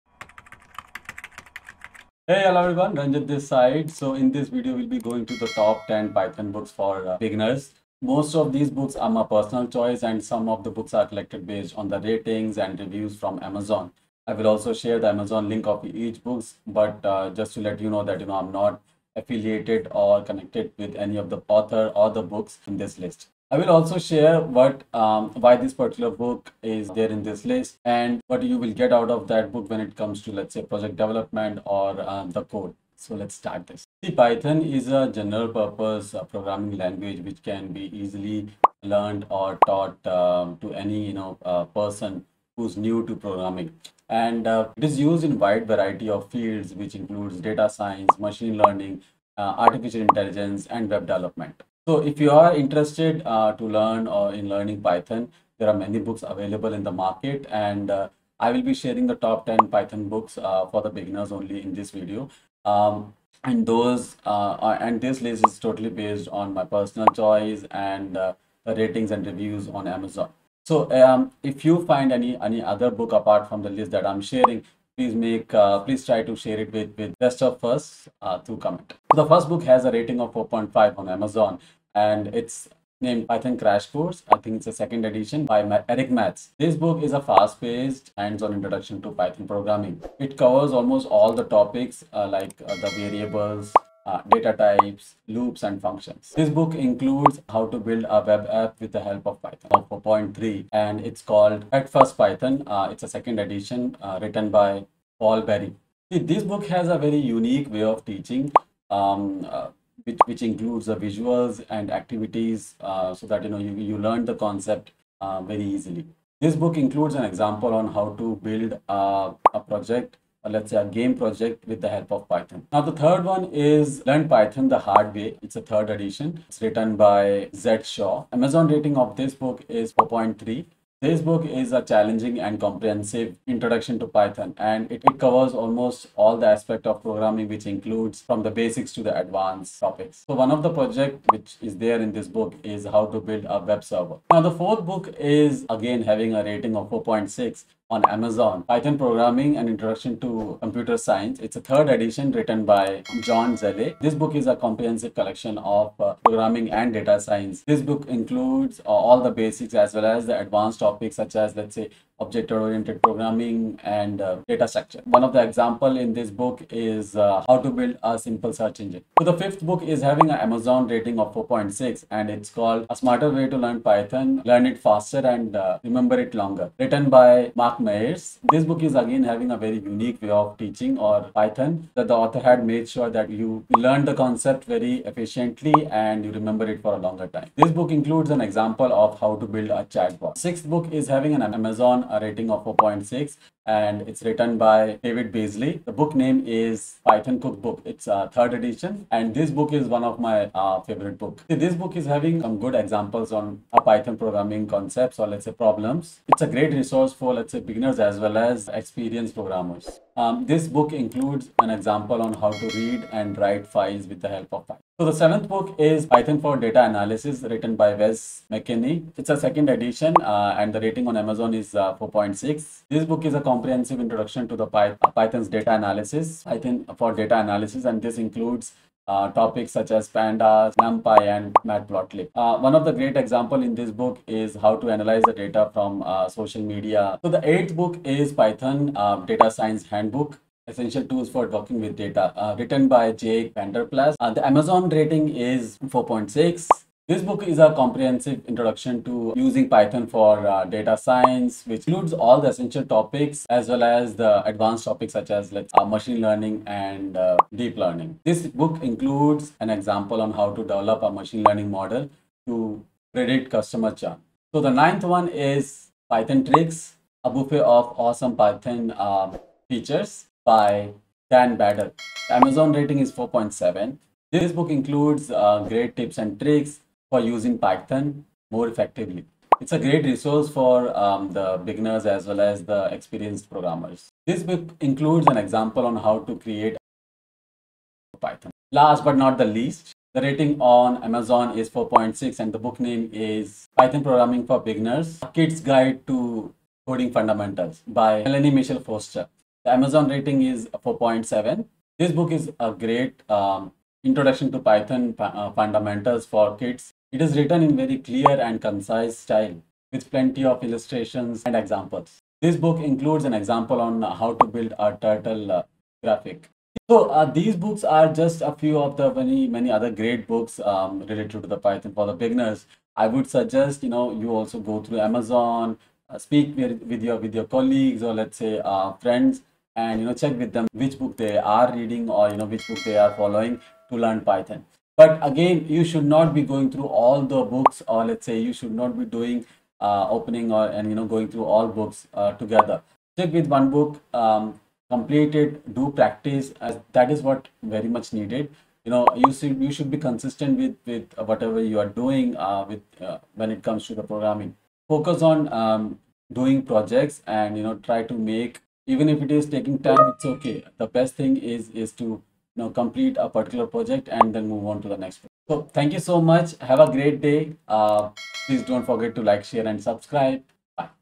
Best Top 10 Python Books list for Beginners | The Data Scientist
Python is a general-purpose programming language that is easy to learn and use. It is used in a wide variety of fields, including webdevelopment, datascience, machinelearning, and artificialintelligence.
If you are interested in learning Python, there are many great books available. In this video, I will share my top 10 picks for the best Python books for beginners, based on Amazon ratings and reviews.

00:00 Introduction
01:19 First Book
01:40 Second Book
02:03 Third Book
02:24 Fourth Book
02:49 Fifth Book
03:10 Sixth Book
03:35 Seventh Book
03:57 Eighth Book
04:26 Ninth Book
04:49 Tenth Book